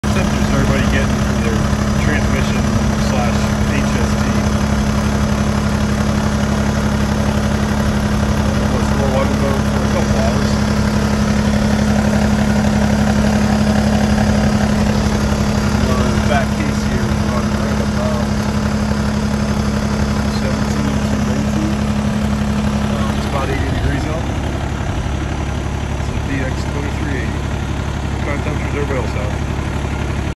Kubota BX Transmission Overheating??? What is Too Hot
What temperature is your transmission running at? I had been mowing for a couple hours near PTO rpm and it was about 80* out. Over 200* seems a little hot but maybe that is normal. The HST fan is in good condition and not damaged. Let me know what you guys think.